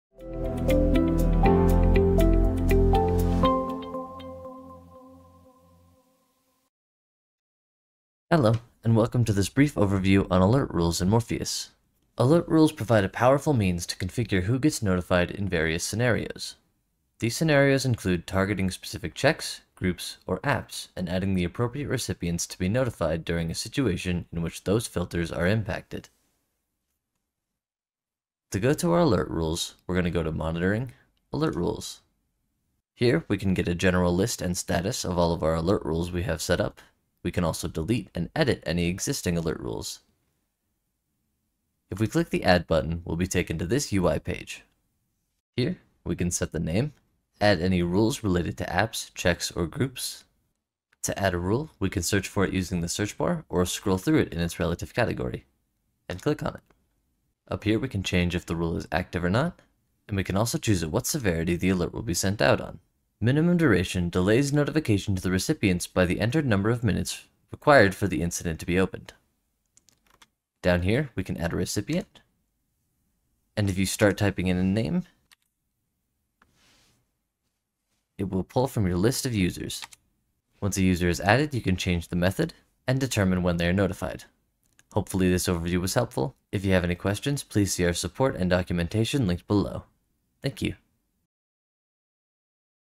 Morpheus Minute Alert Rules HPE
Learn about configuring Alert Rules in Morpheus with this brief overview video. Discover how to set up notifications and manage alerts effectively to enhance your system monitoring capabilities.